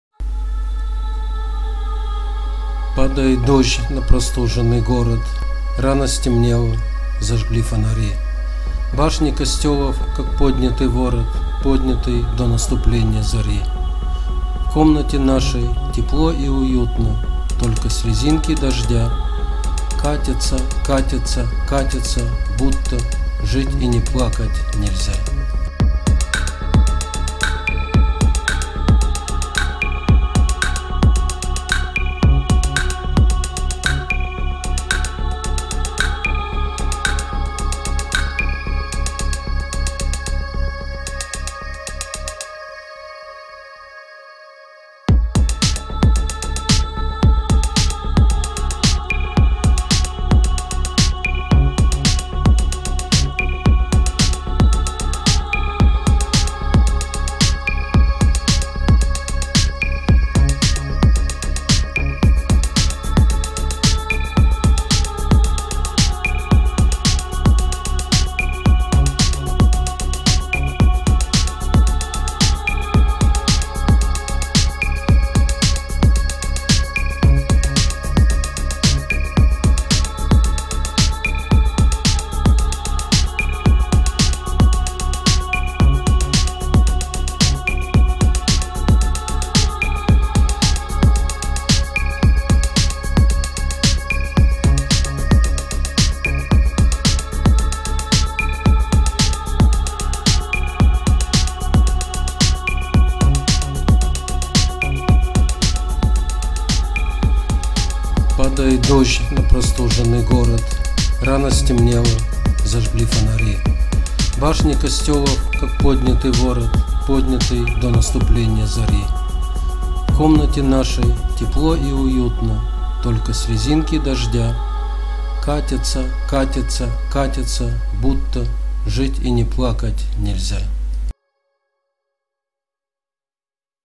Александр Винокур. Падает дождь на простуженный город
Александр Винокур читает стихотворение "Падает дождь на простуженный город".

Поддержите это видео лайком, подпиской на канал, комментарием.

Александр Винокур

Падает дождь на простуженный город.
Рано стемнело. Зажгли фонари.
Башни костелов, как поднятый ворот,
Поднятый до наступленья зари.

В комнате нашей тепло и уютно.
Только слезинки дождя
Катятся, катятся, катятся, будто
Жить и не плакать - нельзя.